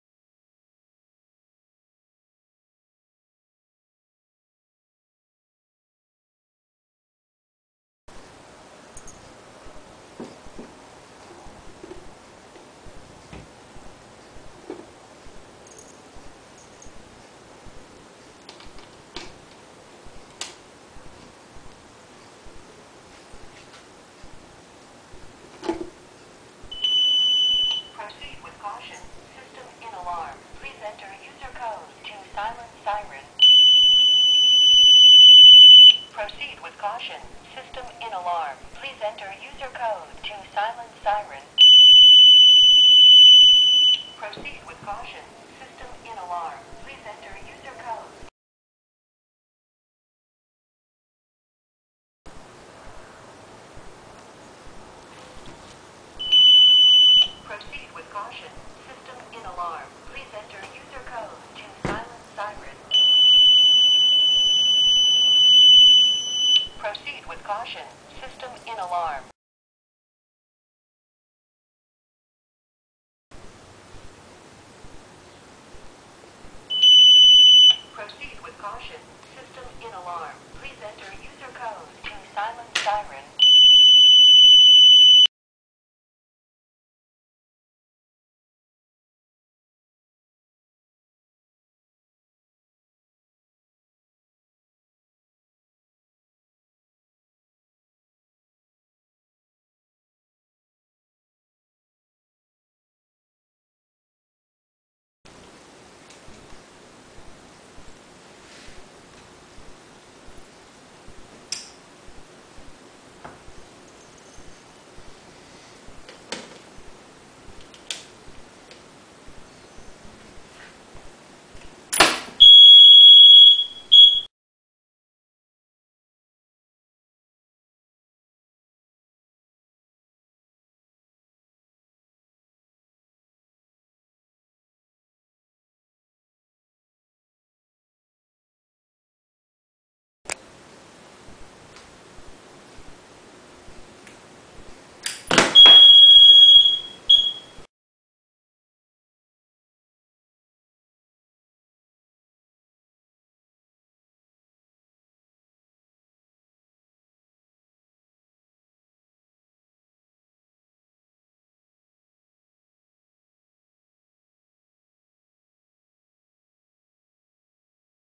The Pistol Pad and Muzzle Switch Video
This video demonstrates The Pistol Pad interfaced to a NAPCO Freedom 64 Security Panel and a Freedom 64 Keypad. Also included in this video is an introduction to the patent pending Muzzle Switch (Coming Soon), which has all the characteristics and benefits of The Pistol Pad, but is designed for shotguns and long rifles!